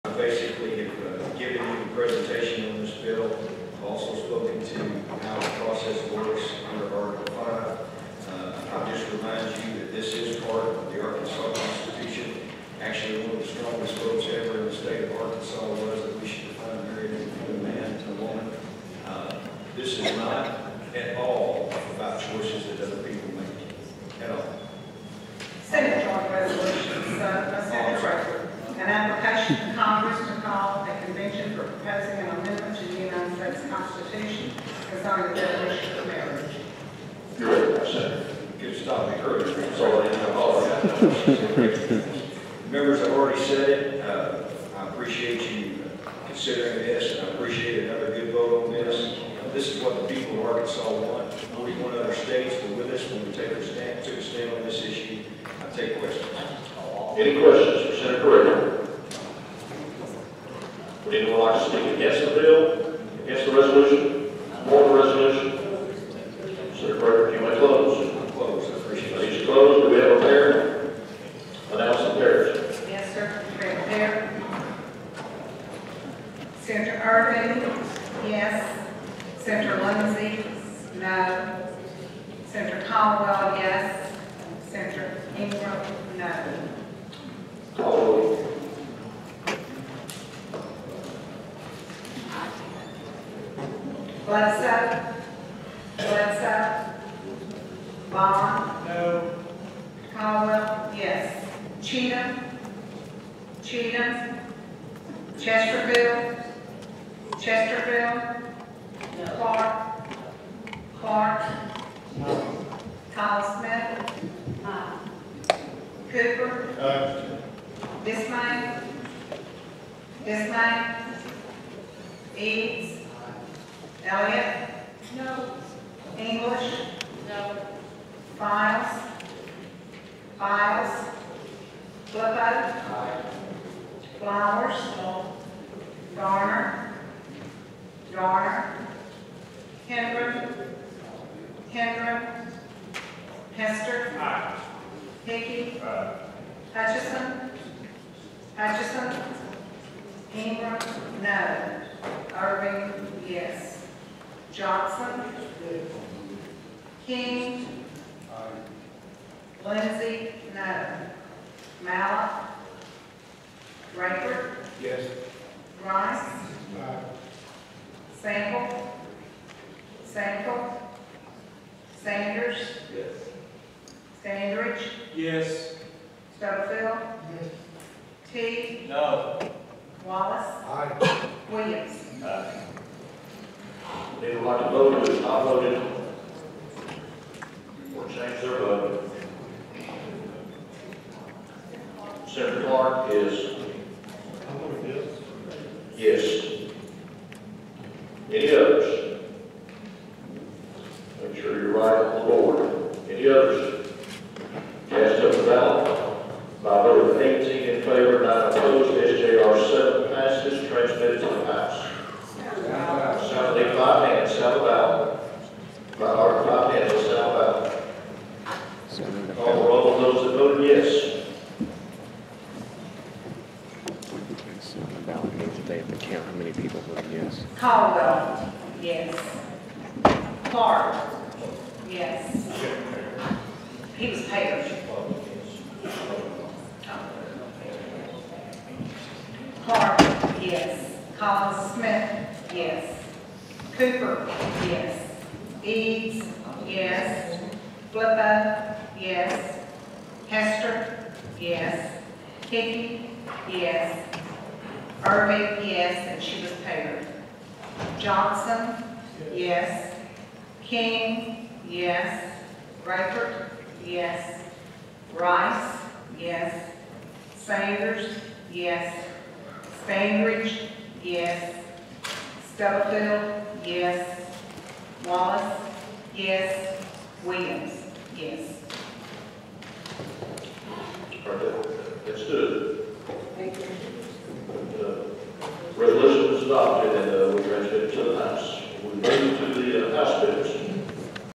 SJR7 Constitutional Convention on Marriage roll call vote and sound the ballot
The Arkansas Senate heard bills regarding a Constitutional Convention of States for Abortion and Marriage.